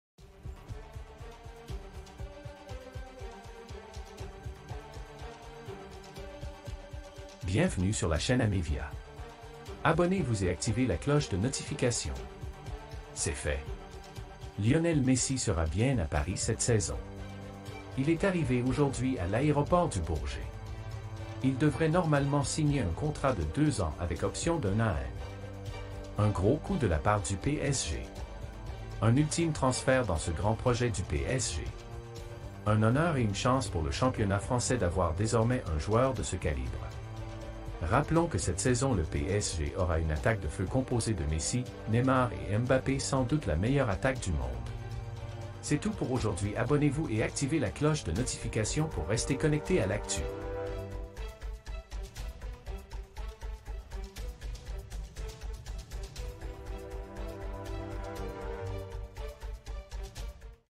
Messi à Paris , c'est fait !
c'est fait  ! Lionel Messi sera bien à Paris cette saison . Il est arrivé aujourd'hui à l'aéroport du Bourgé.
Il devrait normalement signé un contrat de 2 ans avec option d'un an . Un gros coup de la part du PSG. Un ultime transfert dans ce grand projet du PSG. Un honneur et une chance pour le championnat français d'avoir désormais un joueur de ce calibre.
Rappelons que cette saison le PSG aura une attaque de feu composé de Messi , Neymar et Mbappé sans doute la meilleure attaque du monde.